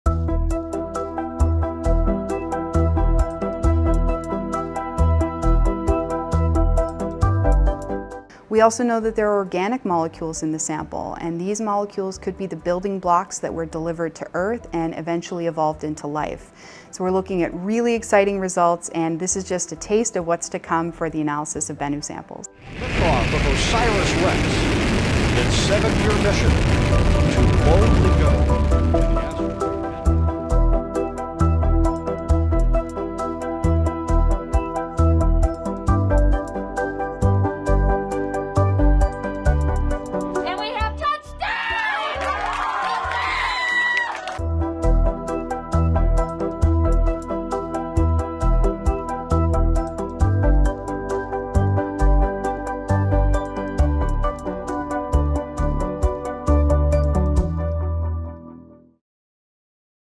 تک‌نگاه - کشف آب و کربن در نمونه سیارک بنو
ناسا می‌گوید نمونه جمع‌آوری شده از سیارک بنو ممکن است سرنخی از چگونگی آغاز حیات روی زمین به دست دهد. این نمونه که توسط کاوشگر اوریسیس رکس از سیارک بنو جمع‌آوری شد یک ماه پیش به زمین رسید.